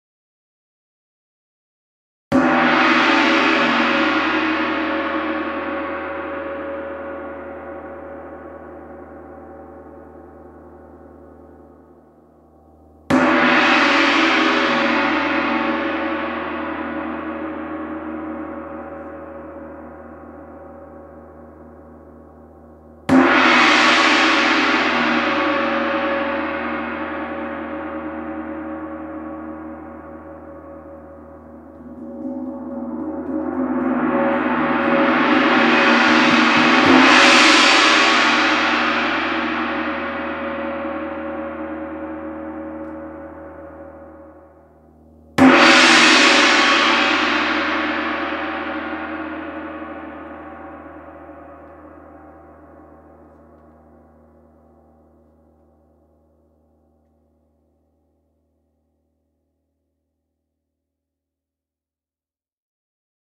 WHD 32in Chau Gong + Adjustable Stand
The Chau Gong, also known as the Bullseye Gong, Chinese Gong and Tam Tam produces an explosive crescendo full of warm, deep tones and with the iconic bullseye design it looks every bit as good as it sounds. Also included is a high grade, lightweight steel gong stand, beater and hanging strap making this convenient bundle exceptional value for money.